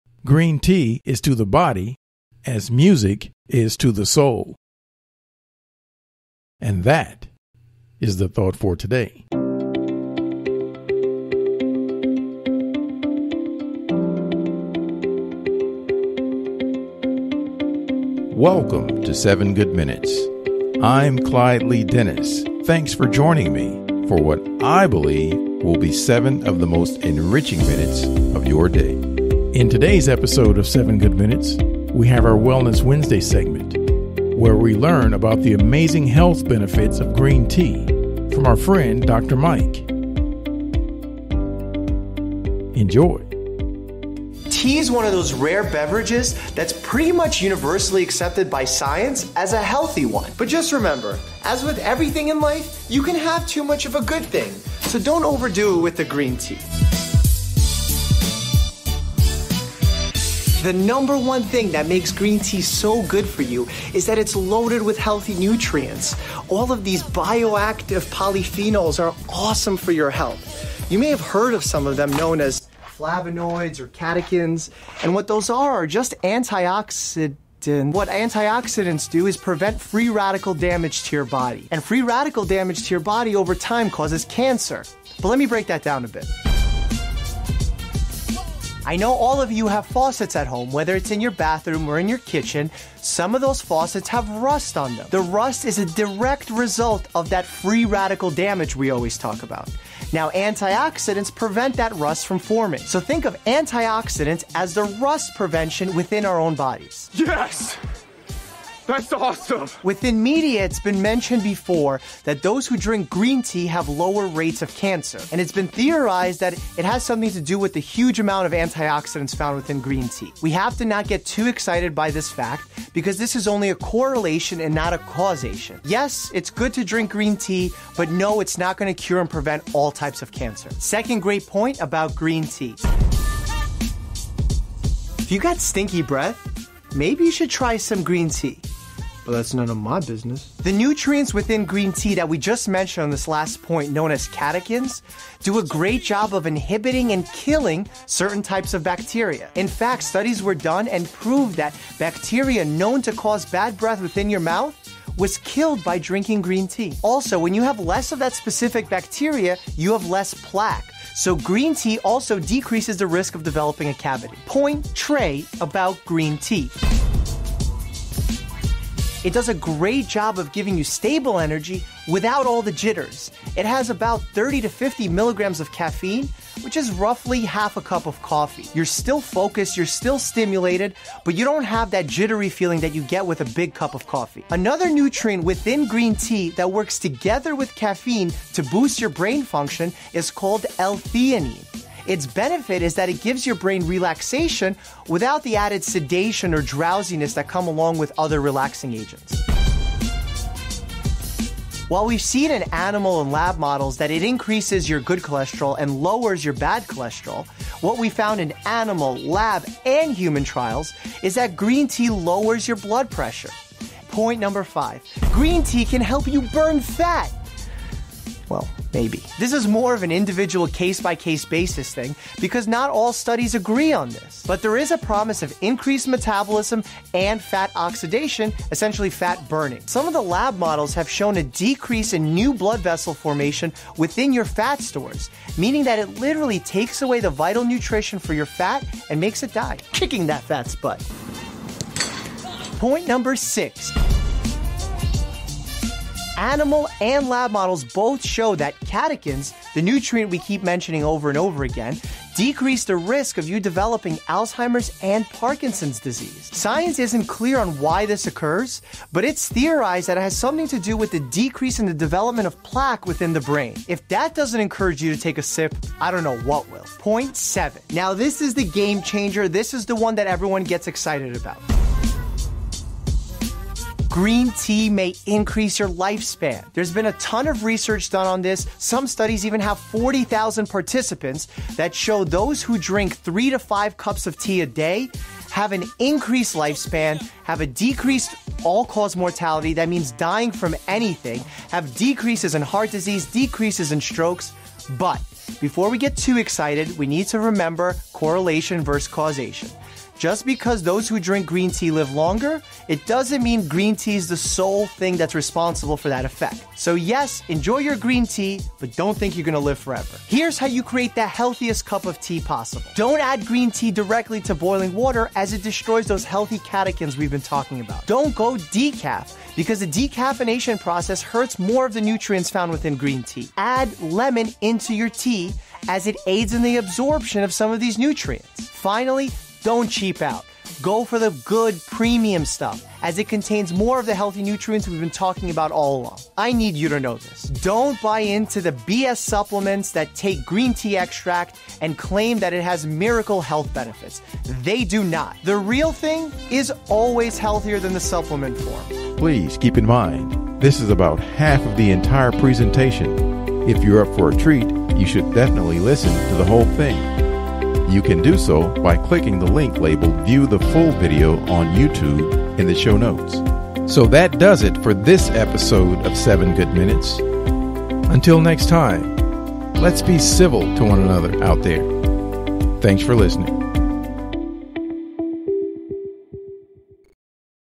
Wellness Wednesday: The Amazing Health Benefits of Green Tea
In this episode of the 7 Good Minutes Podcast, we have our Wellness Wednesday segment where we learn about The Amazing Health Benefits of Green Tea from our friend Doctor Mike.

The audio clip featured in today's episode of the 7 Good Minutes Podcast is courtesy of our friend Doctor Mike

The Amazing Health Benefits of Green Tea

► How To Walk Correctly For Better Health and Fitness